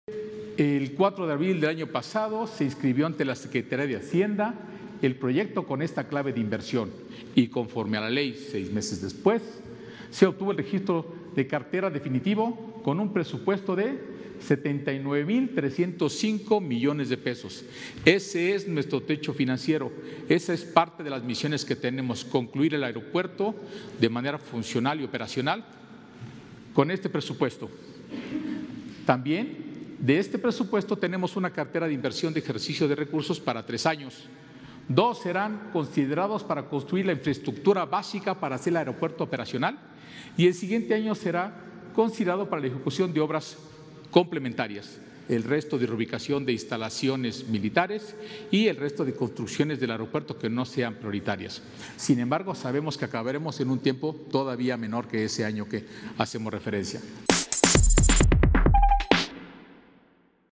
Presupuesto para el Aeropuerto fue de 79 mil 305 mdp
El presupuesto autorizado por la Secretaría de Hacienda para el Aeropuerto Internacional «Felipe Ángeles» es de 79 mil 305 mdp, con la misión de «concluir el aeropuerto de manera funcional y operacional», señaló el comandante Ricardo Vallejo.